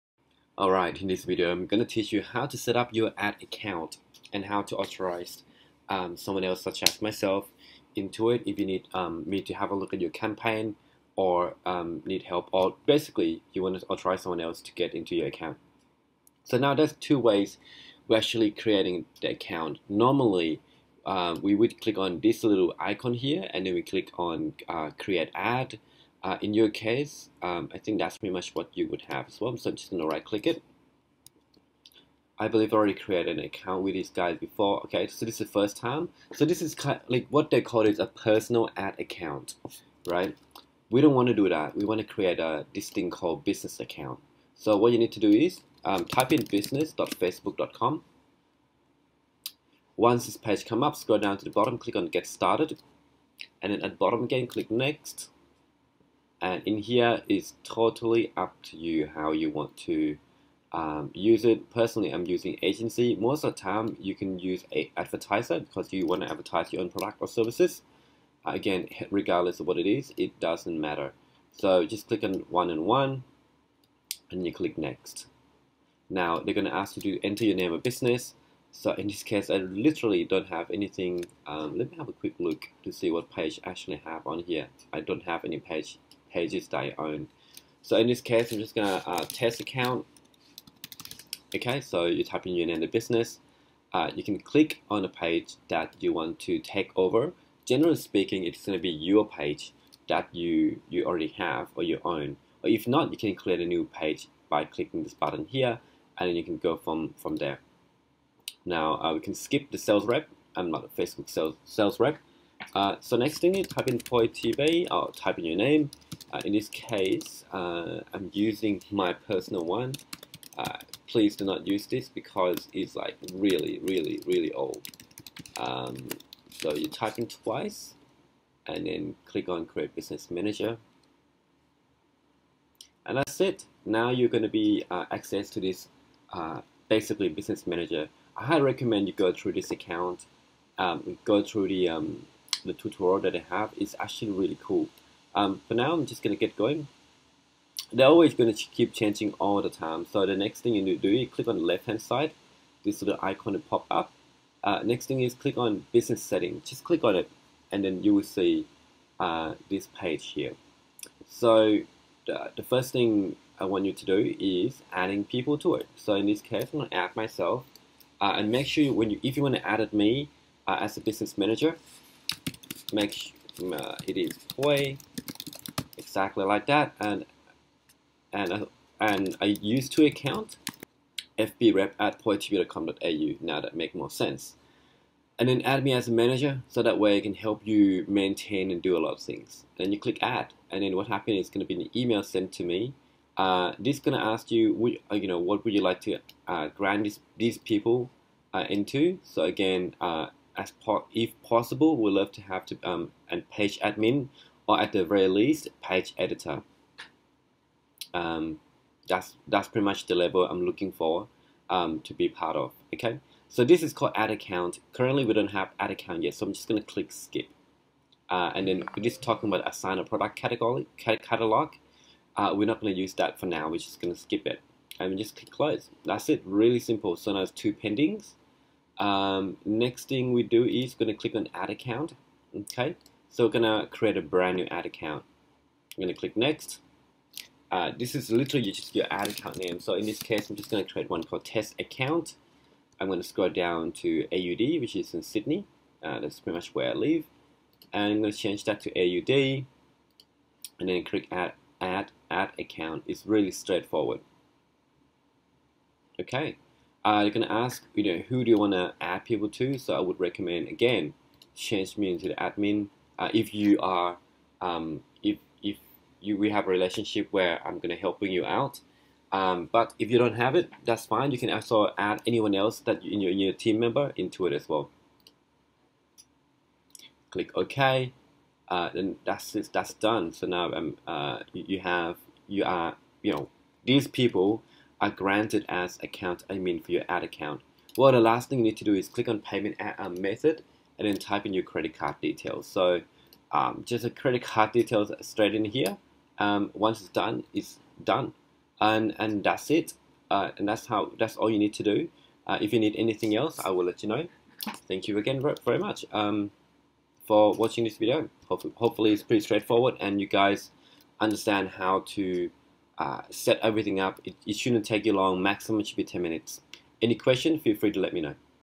How to setup Facebook Business Ad account
Learn How to setup Facebook Business Ad account

In this quick video, I teach you how to setup Facebook Advertising account correctly for your business so that way you can invite others workers and people to help you out on your advertising campaign using Facebook Business rather than a normal personal advertising platform.

Please note, this video was dedicated to my clients so I have my personal email address in the videos for me to assist with them one on one or do advertising for them.

and download how to advertise on Facebook for FREE!